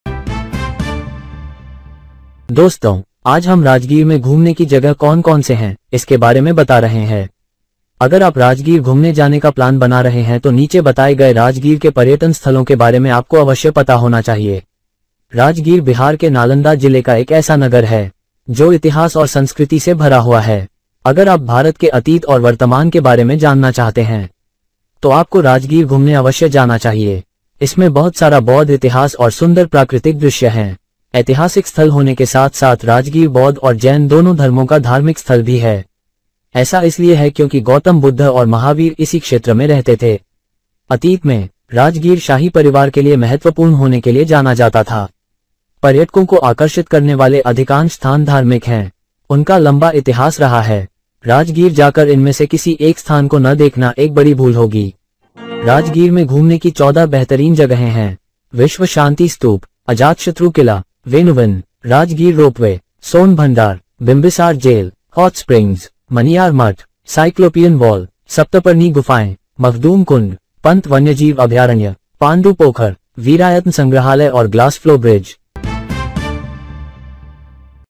Main tourist places of Rajgir || राजगीर के प्रमुख दर्शनीय पर्यटन स्थल
Nalanda was an ancient Buddhist center of learning and one of the world's first universities, located in Bihar, India. It was founded in the 5th century CE and flourished until the 12th century CE. It was a major center of Buddhist studies and a hub of intellectual and cultural activity, attracting scholars and students from across Asia. The ruins of the university are a UNESCO World Heritage Site.

Now Nalanda is both a district the state of Bihar, India. The district is named after the ancient Buddhist center of learning, Nalanda University, which was located in the area. The district is located in central Bihar and is known for its rich cultural and historical heritage, including the ruins of the ancient university. The district headquarters is located in the city of Bihar sharif.